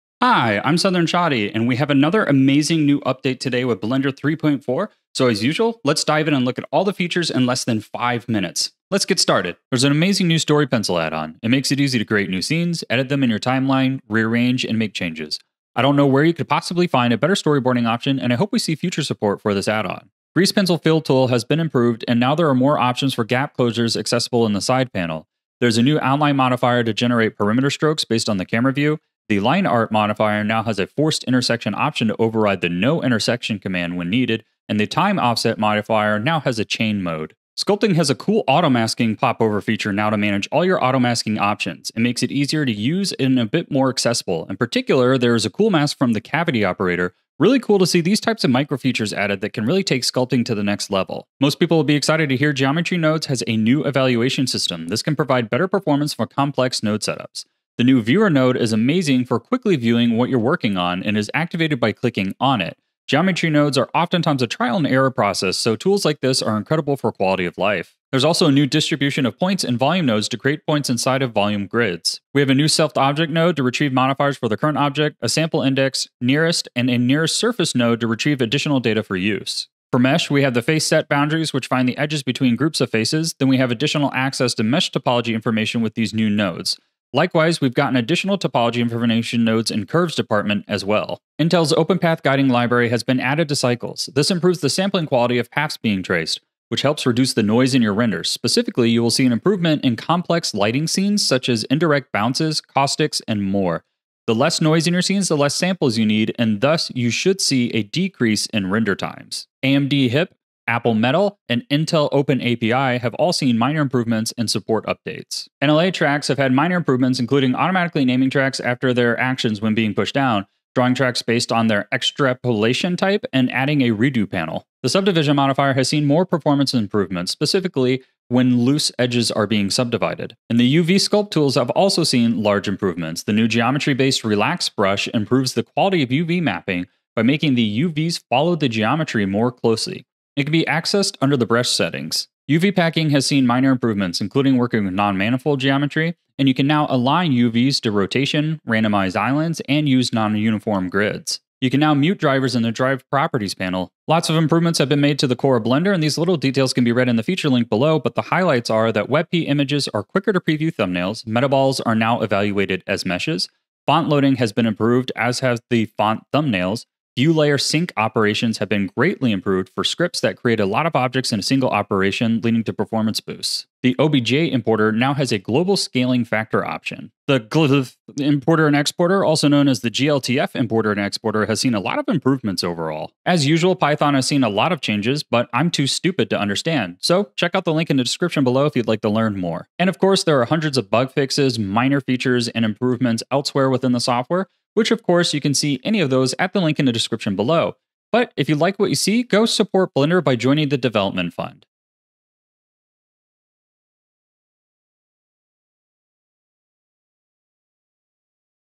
Blender 3.4 New Features in LESS than 5 minutes
Check out the full list on what's new in Blender 3.4

Video host: @SouthernShotty